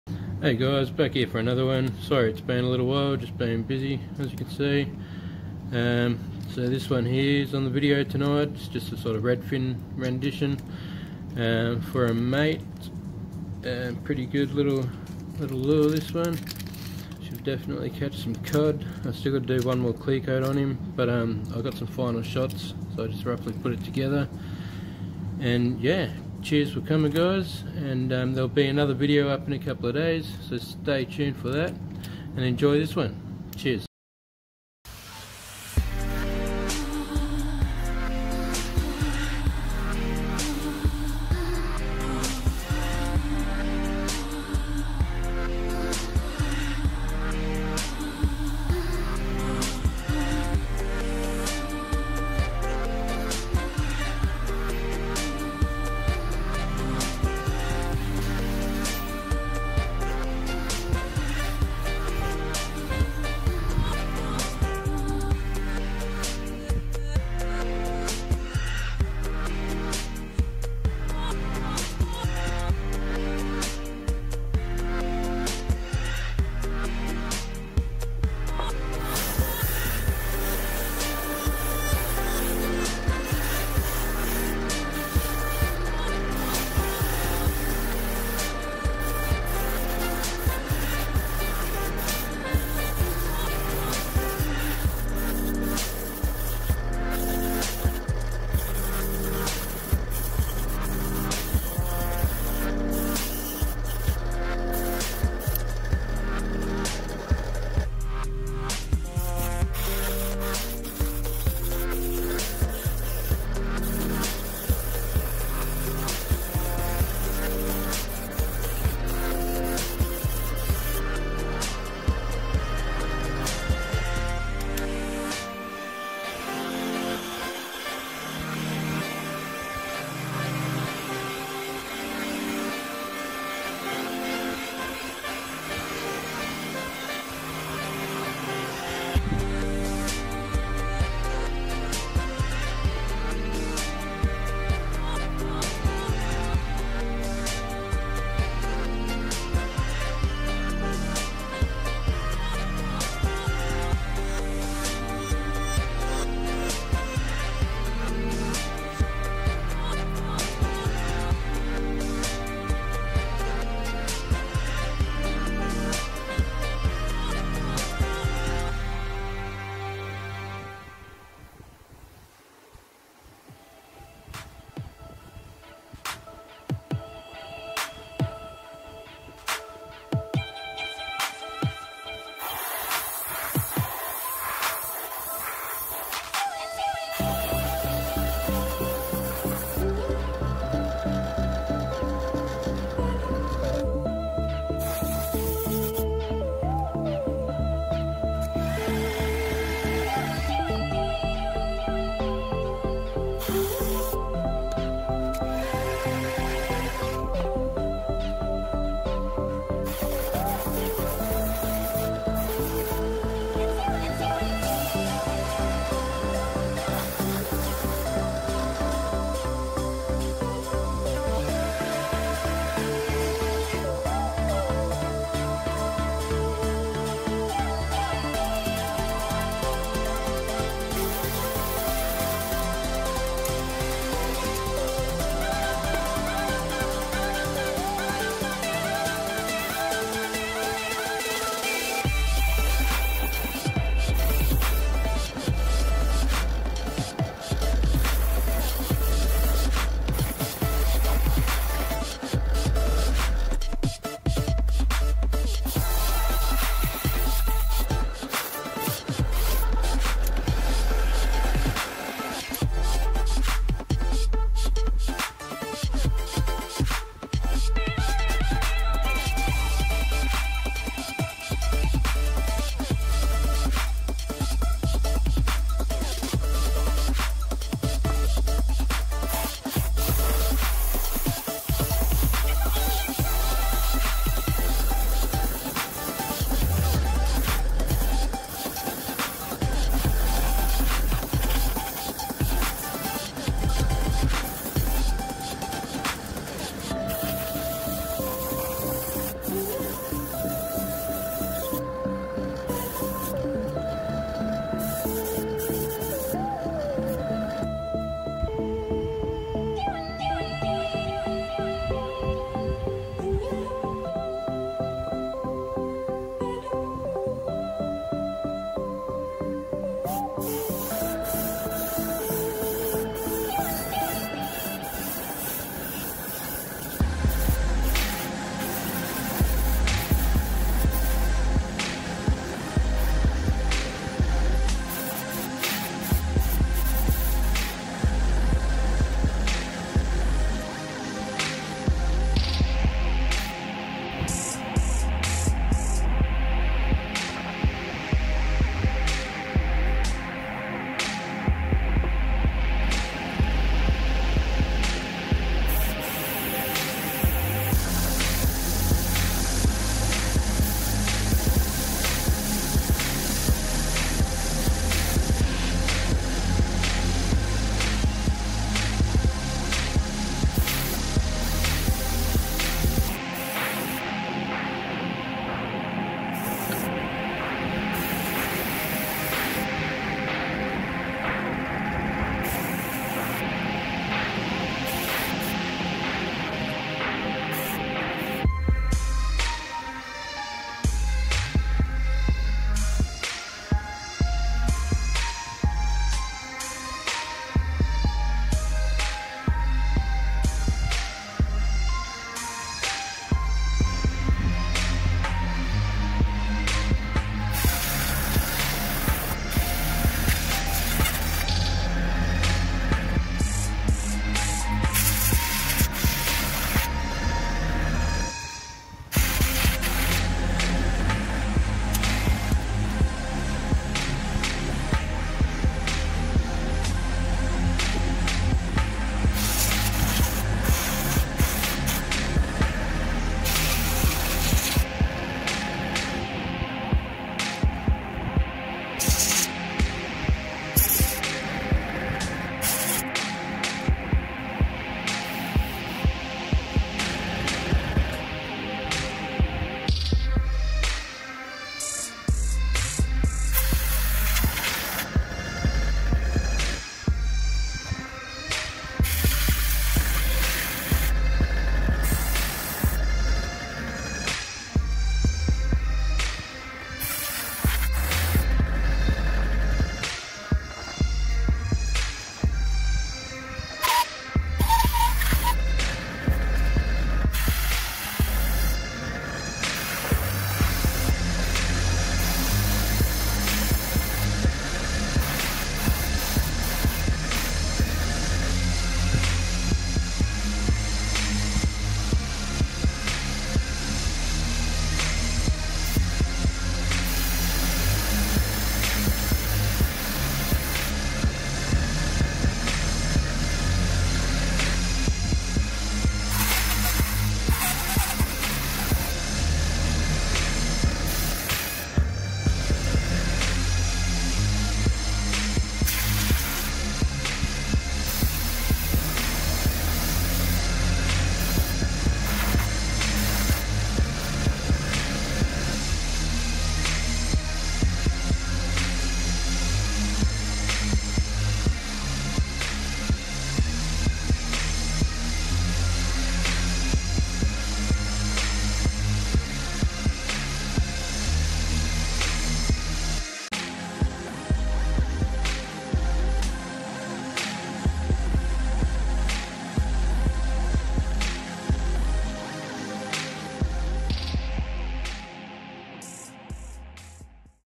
Custom Redfin Perch Swimbait (Lure Painting)
In this video I custom paint one of my handcrafted timber lures in a 'redfin perch' pattern using a range of Outlaw Paints. Its not a completely realistic version, but I know these colours will get smashed.
This is one of my new lures, still working on some different varients before released on out wesite. with a fantastic top water column swim action and ability to jump over sticks, this all terain swimbait will be great to target Murray Cod.
These will be available on our website throughout the year...

Dont forget to check out Outlaw Paints amazing ever growing range at